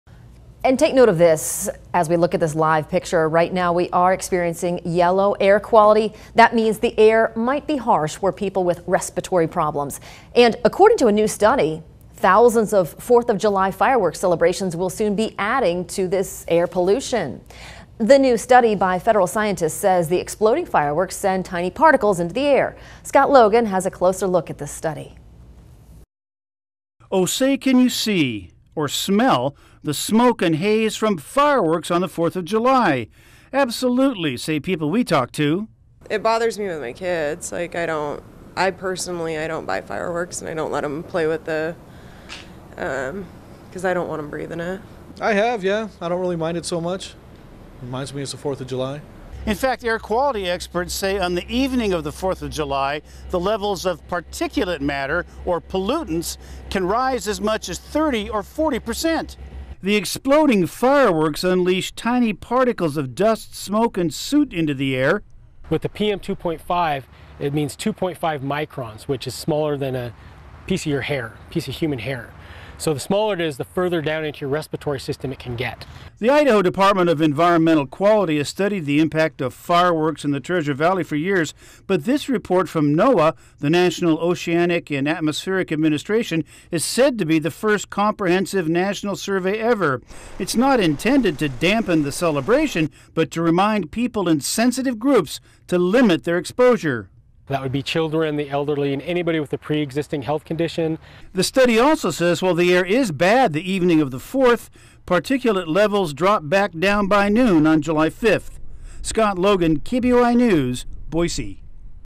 Already at Yellow Alert, Fireworks May Worsen Ada County Air Quality
Ada County is already at a yellow air quality alert and this weekend's fireworks are not expected to help.